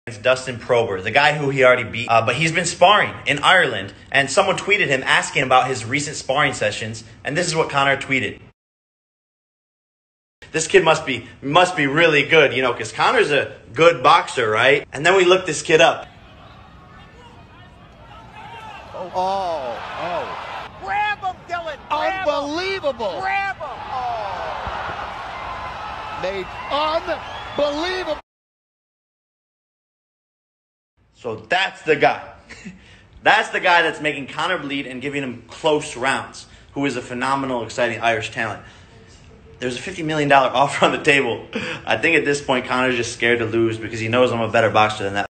Jake Paul mocks Conor mcgregor and his sparring partners
Jake Paul calls out mcgregor again saying he is a bad boxer and his sparring partners can’t box and that he’s scared of him